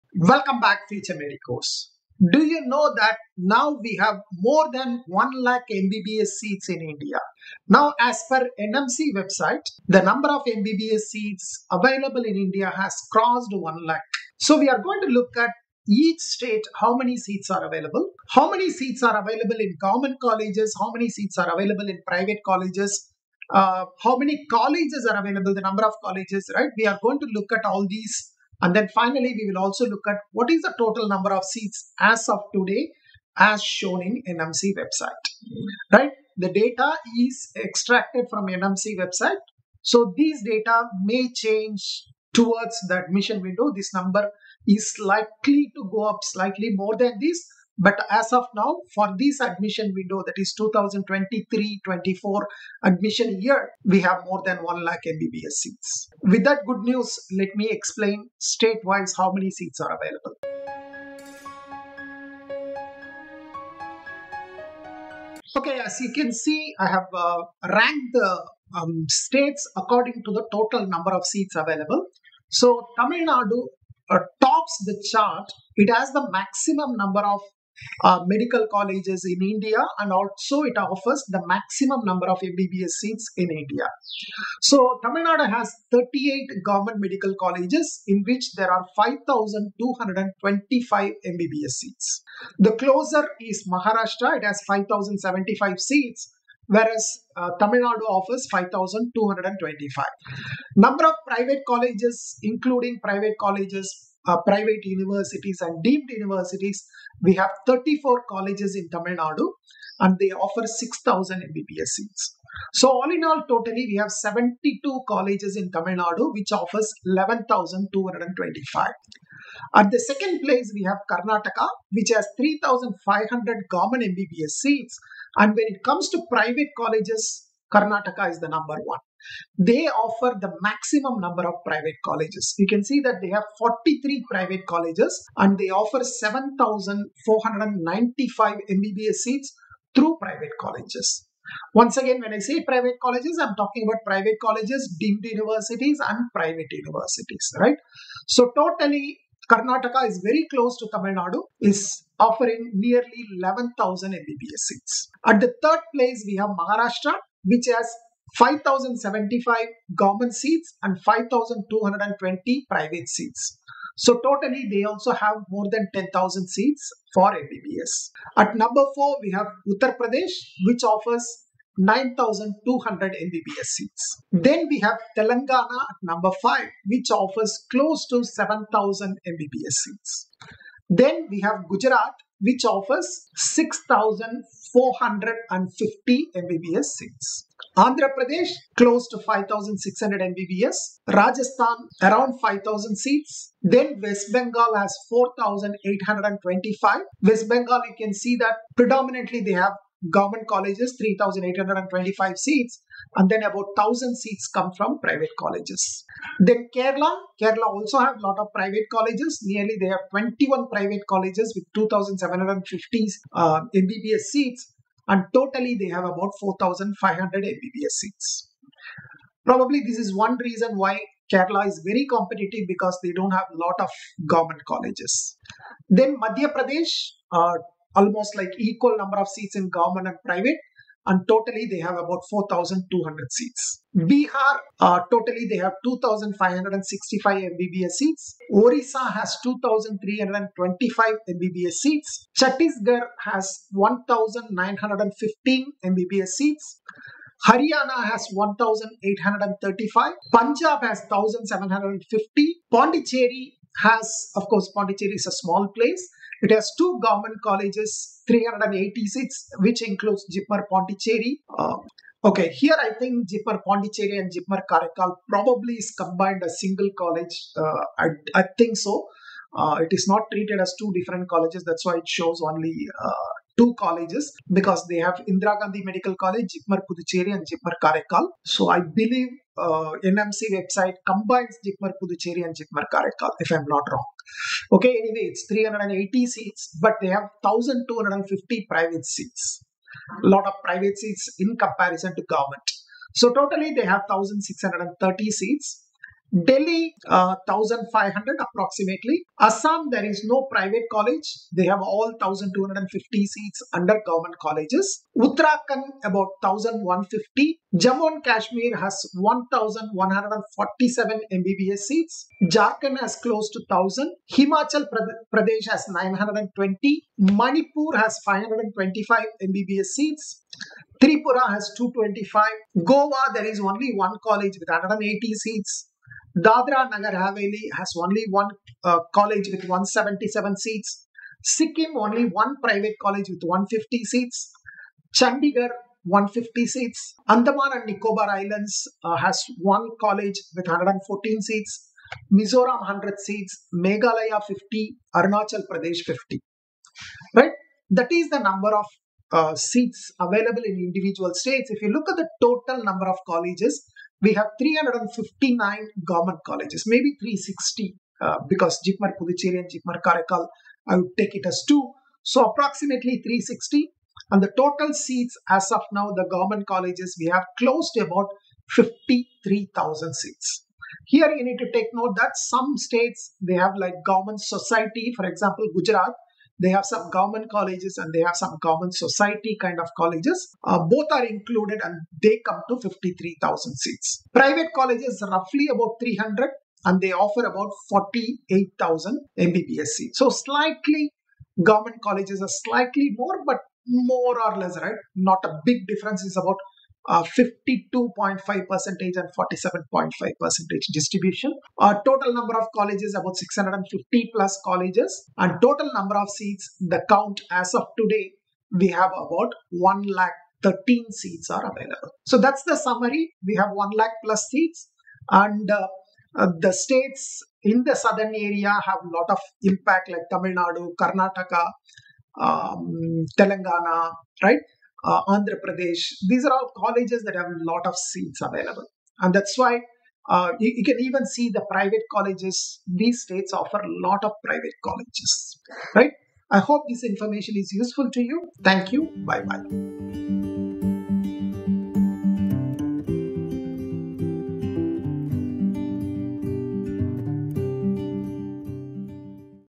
1 lac MBBS Seats in India 🔥🔥🔥
Do you know that now we have more than 1 lac MBBS seats in India? In this video, I have given the state-wise breakdown of the 1 lac MBBS seats. You will come to know the number of government colleges and number of private colleges in each state. You will also understand the number  of government MBBS seats and number of private MBBS seats in each state. Here, private colleges refer to private medical colleges, private universities and deemed universities.